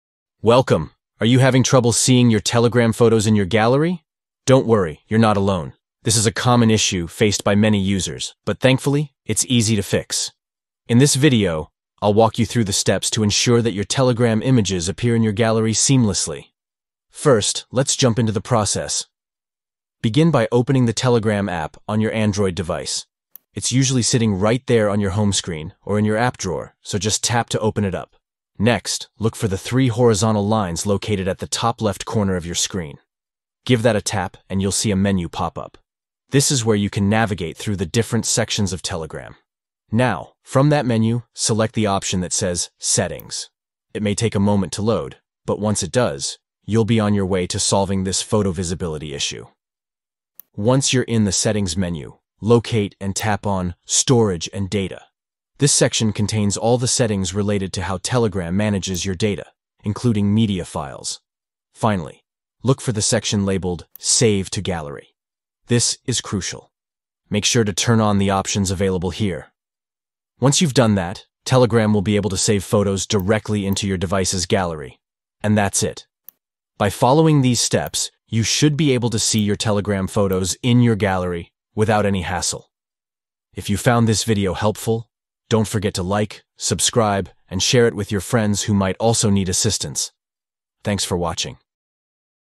How to Fix Telegram Photos Not Showing In Gallery (Updated)
Learn how to fix the issue of Telegram photos not showing in your gallery with this easy-to-follow guide. No more missing images or frustrating moments!

Steps:
1. Open Telegram on Your Android device.
2. Tap the three lines at top left.
3. Tap "Settings".
4. Tap on "Storage and Data".
5. Under "Save to Gallery", turn on the options.

Now you can enjoy your Telegram photos in your gallery without any issues.
Chapters:
00:00 Intro
00:12 Soultion
01:05 Outro